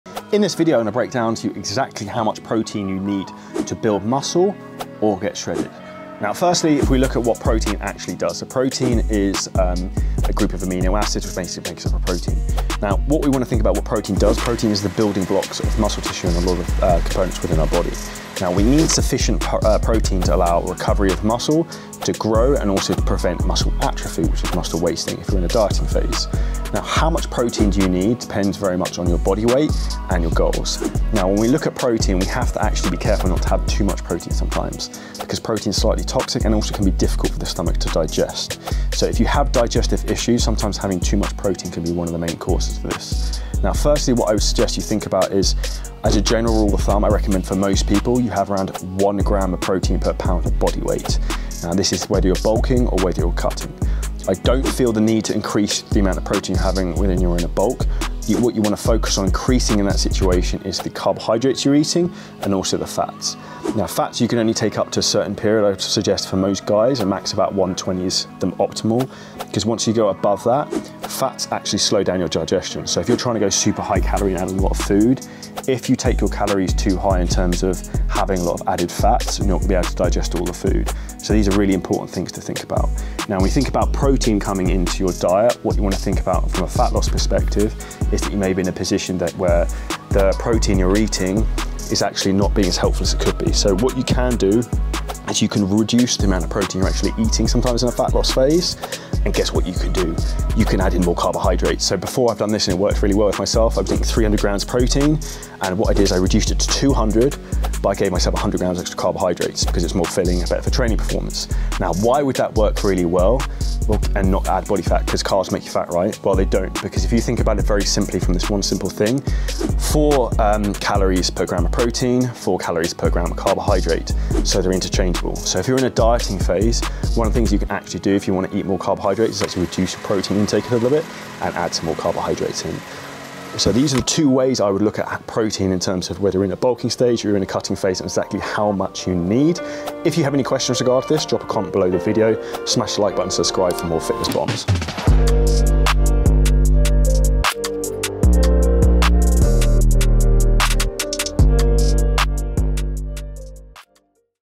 How Much Protein Do You Need to Bulk Up or Get Lean
Unsure on how much protein you should be taking? What are you goals?
Protein intake is crutial to both building muscle and getting yourself lean with a six pack.

Watch along as I give you the answer to your body transformation success.